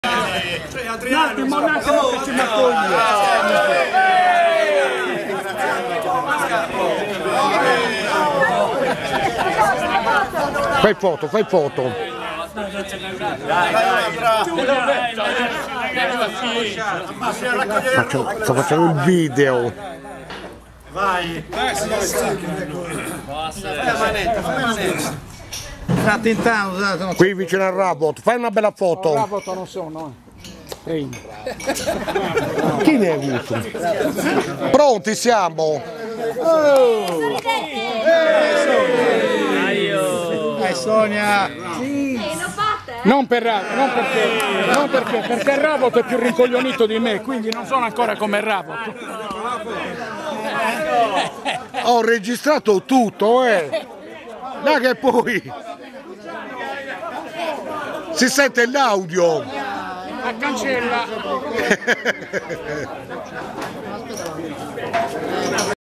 EX CALCIATORI GRANATA DEL S.ANTIOCO CALCIO
Giornata trascorsa da Sisineddu a Cussorgia con amici di vecchia data tutti ex giocatori di calcio isolano.